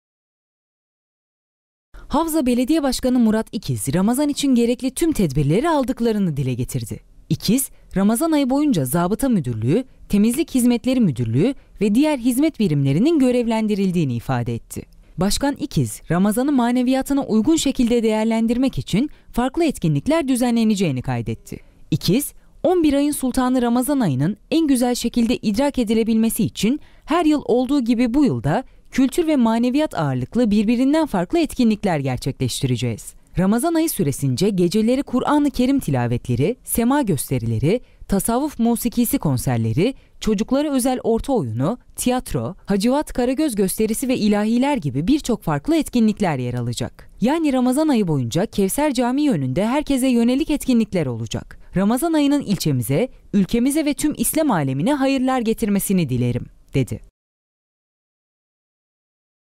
Tüm tedbirler tamam
Havza Belediye Başkanı Murat İkiz, vatandaşların Ramazan aynı rahat ve huzurlu bir şekilde geçirmesi için gerekli tüm tedbirlerin alındığını belirtti.